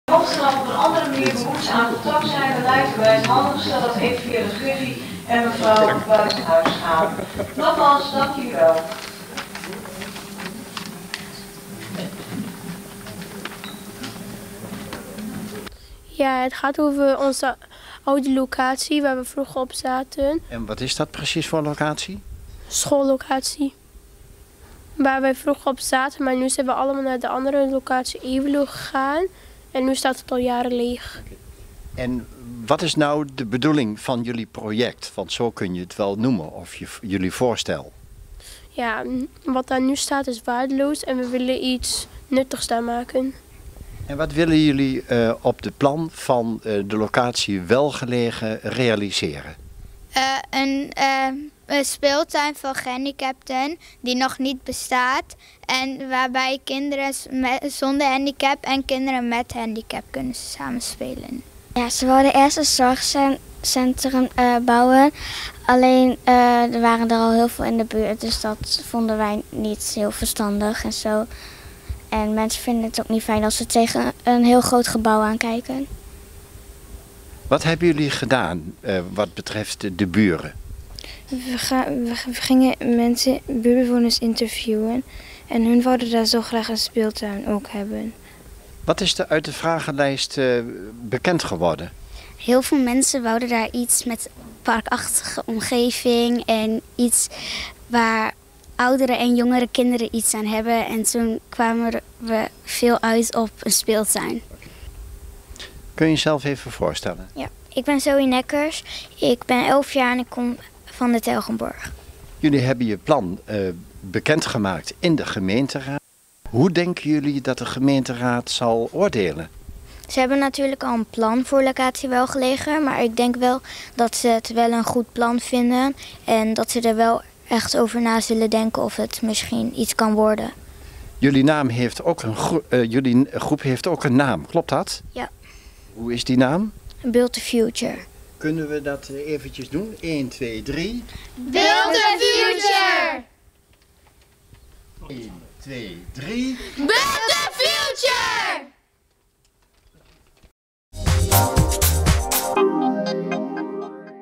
Leerlingen Telgenborch ontwerpen Samenspeeltuin
Zes leerlingen van basisschool De Telgenborch maakten in de raadszaal hun plan voor de Samenspeeltuin bekend.

Volgens hen verloedert de locatie 'Welgelegen', de oude school voor protestant-christelijk onderwijs die vanwege een dalend leerlingenaantal al een paar jaar geleden is afgestoten. Die verloedering is de leerlingen en de buurtbewoners een doorn in het oog. Als onderdeel van de First Lego League werden ze uitgedaagd om de maatschappelijke rol van techniek en technologie te onderzoeken.

Ze kozen voor een speeltuin waar gehandicapte en niet-gehandicapte kinderen samen kunnen spelen. Voor het beoogde zorgcentrum op die locatie is volgens de leerlingen geen plaats: die zijn er in de omgeving al genoeg. Bovendien neemt de drukte door autoverkeer alleen maar toe. Een groot gebouw past volgens de leerlingen ook niet in de omgeving.